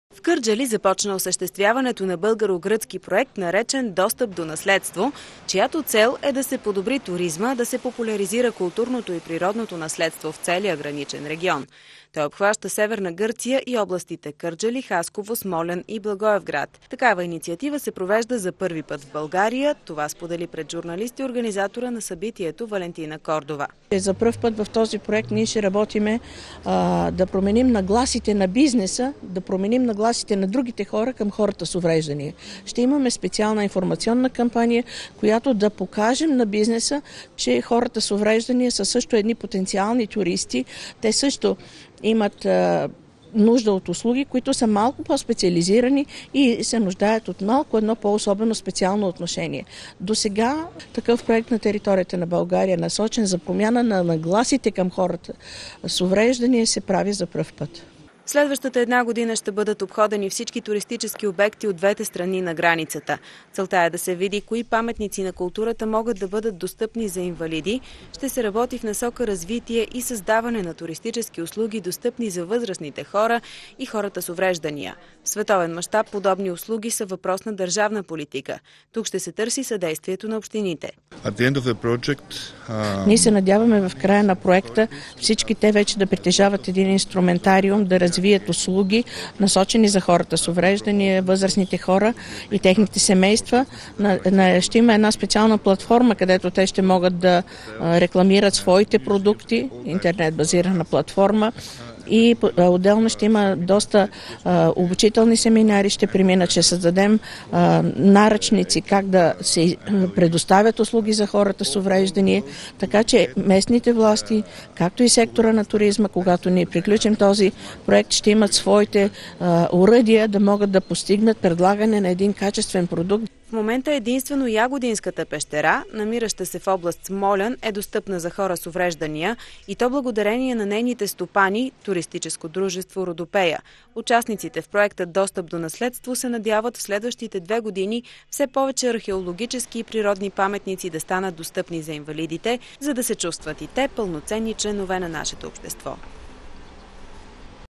Проект за достъпна туристическа среда за хора с увреждания
В Кърджали започна осъществчването на българо-гръцки проект, наречен “Достъп до наследство“. Неговата цел е да бъде създадена достъпна туристическа среда за хората с увреждания